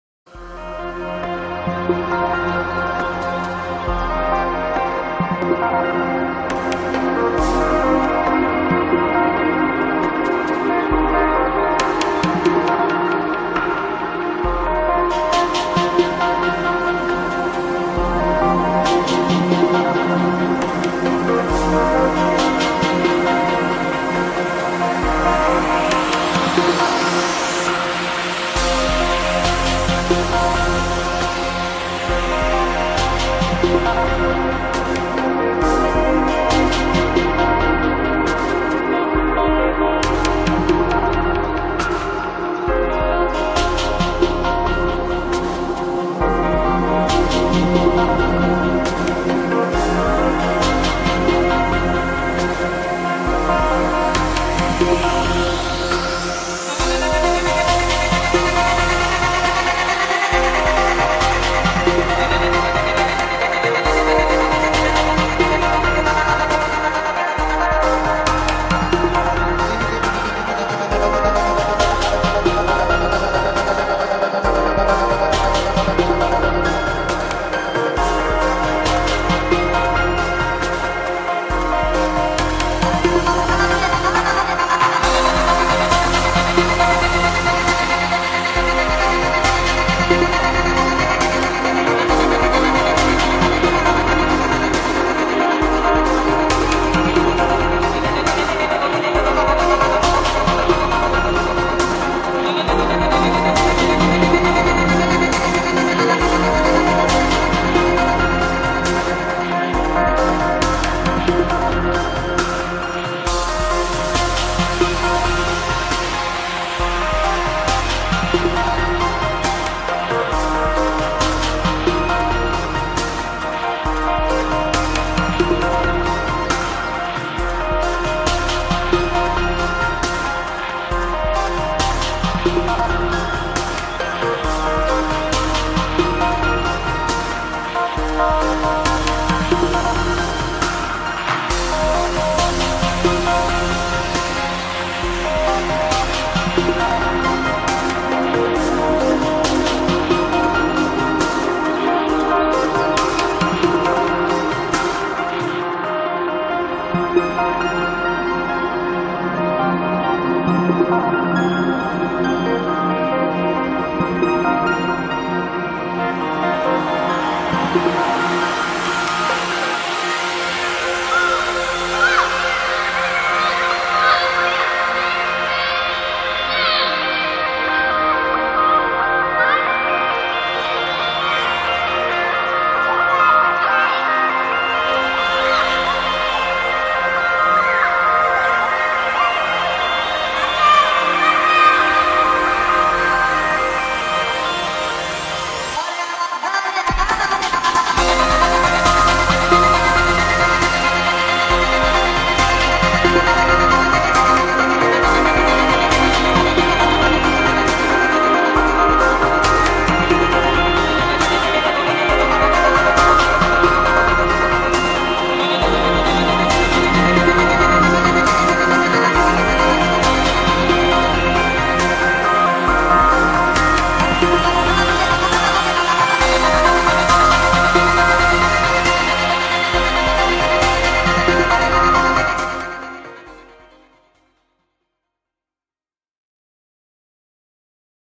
First day in Copenhagen
my first day in Denmark
all videos and photos are unedited and were shot with GoPro Hero 5 Black